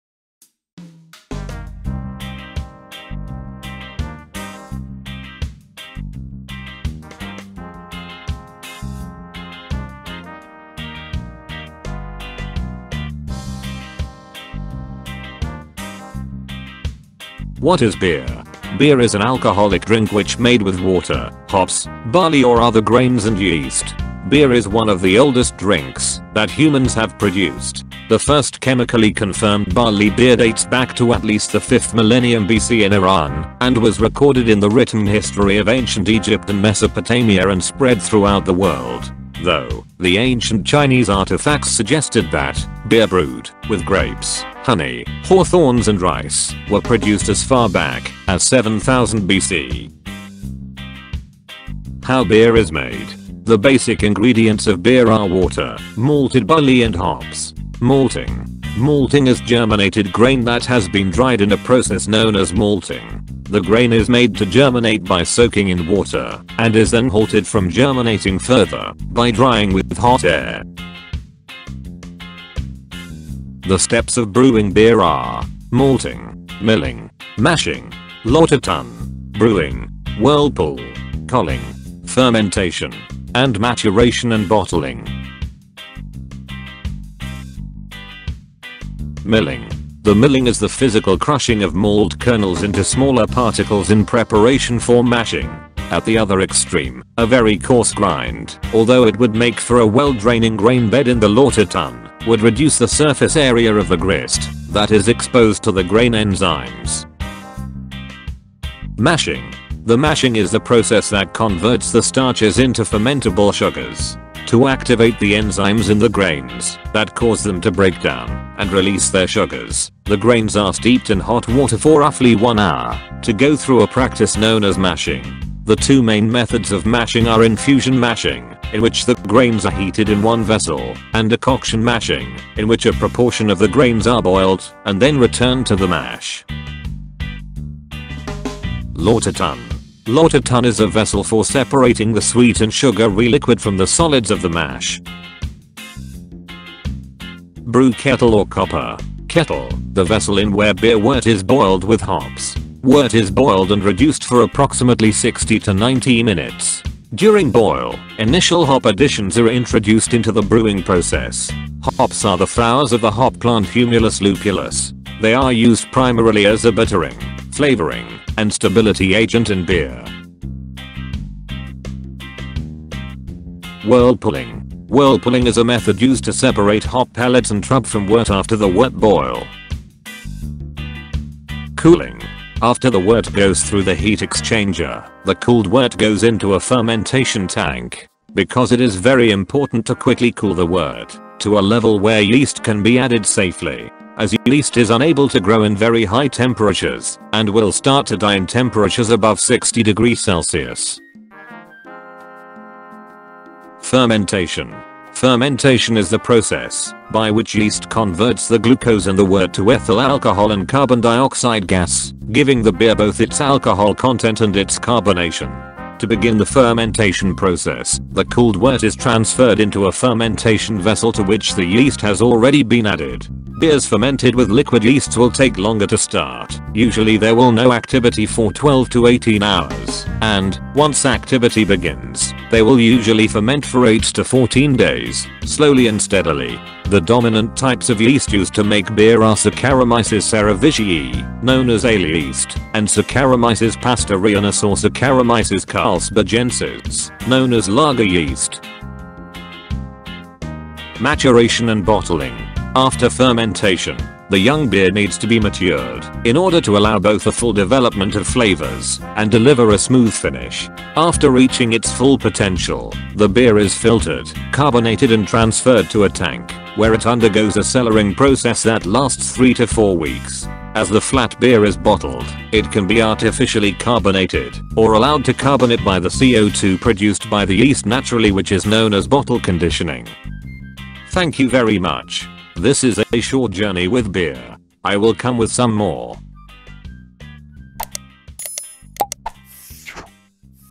How is Beer made? What Is Beer? | The Beer Making Process
A short history and brief description about Beer and its making process like
Malting
Milling
Mashing
Lauter Tun
Brewing
Whirlpool
Colling
Fermentation
Maturation
and also about beer-yeast ,hops.